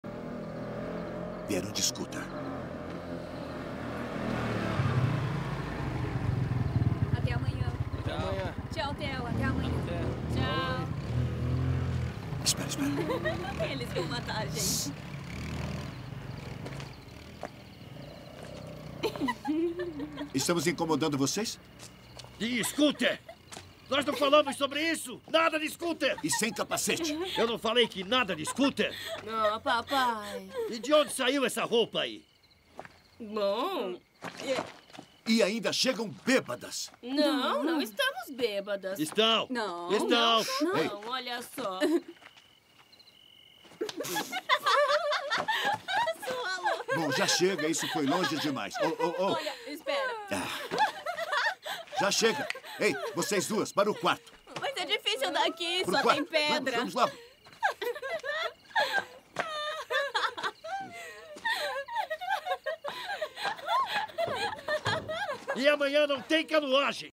Reação dos pais ao verem as filha bêbadas
Exemplo de uma situação social